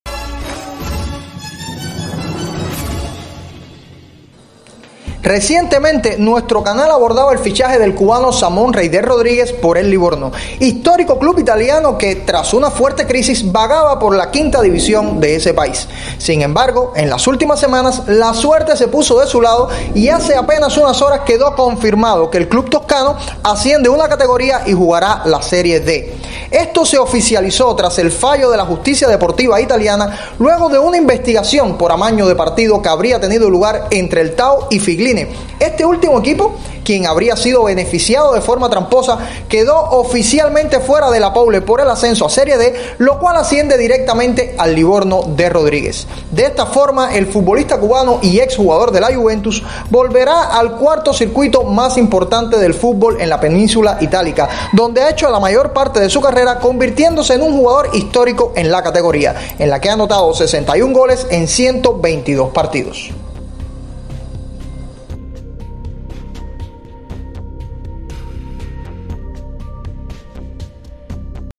Cubano Samón Rodríguez volverá a Serie D con el Livorno tras escándalo por amaño de partidos
Mira como un ESCÁNDALO por AMAÑO de partidos le da el ascenso al futbolista CUBANO Samón Reider Rodríguez, que recientemente había fichado por el Livorno.

❗Únete a la familia GolCuba en redes.